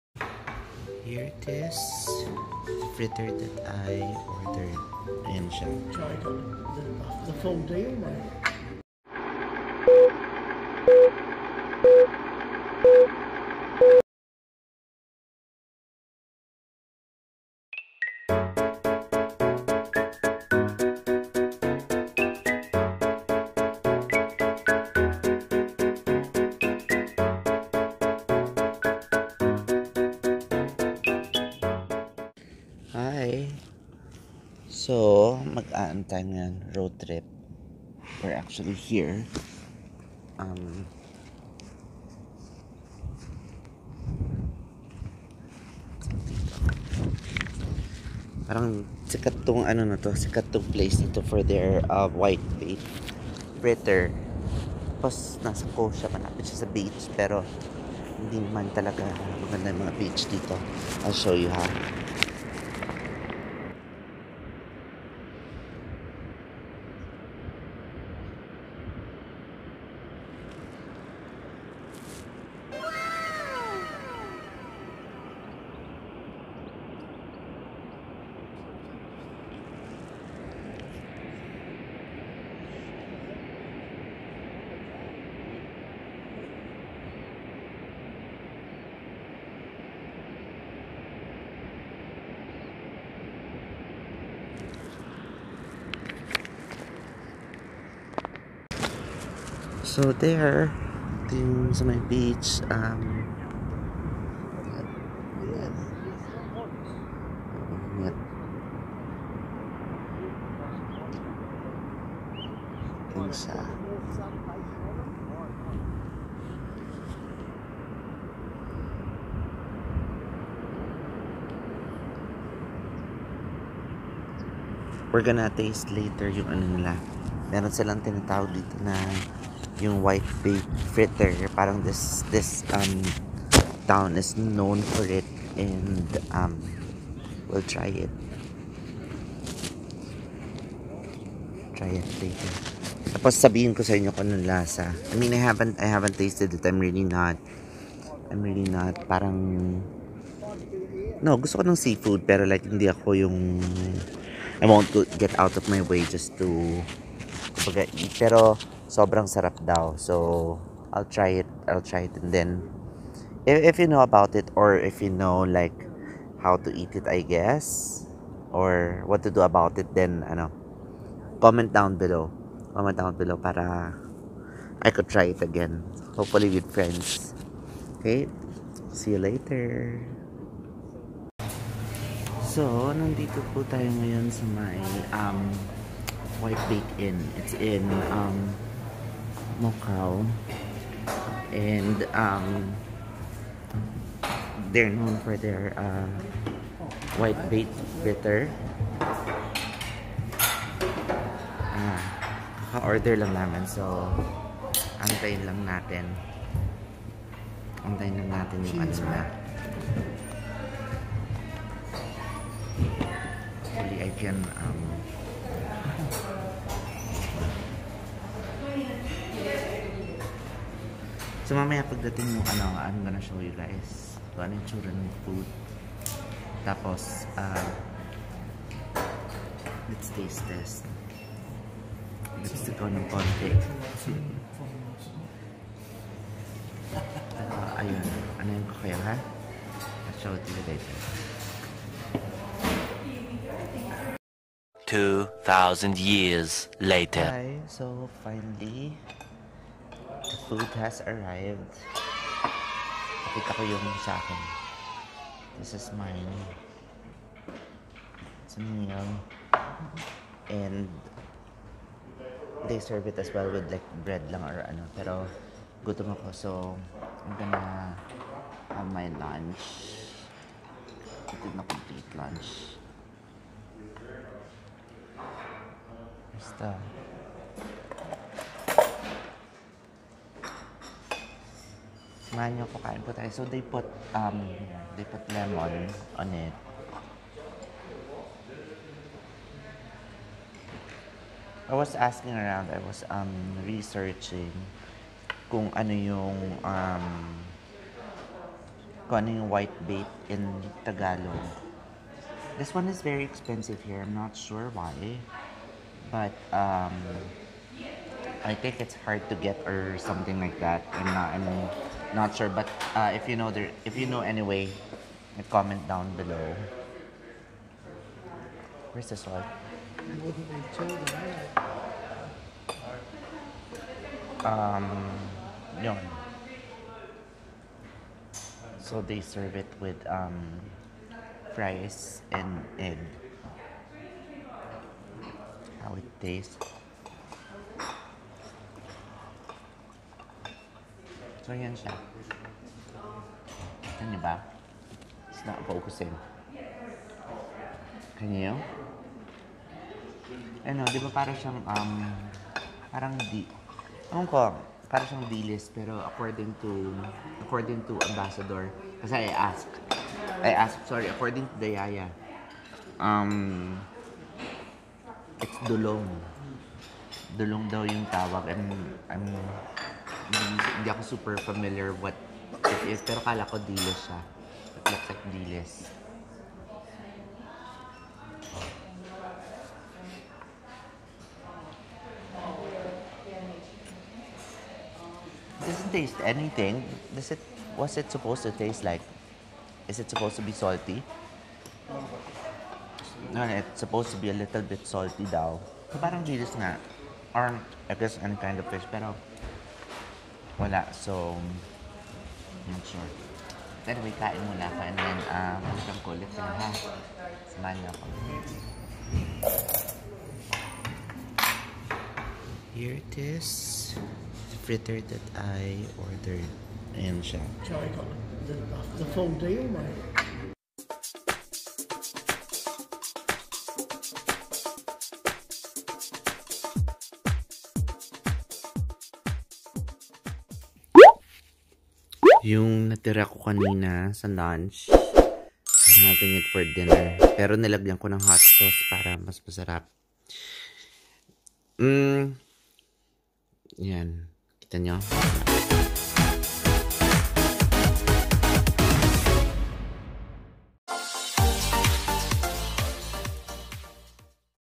WHITEBAIT MOKAU || FILIPINO EXPERIENCE NEW ZEALAND || IAMJOEYVEE
Whitebait fritter in mokau.
Best place to experience this food.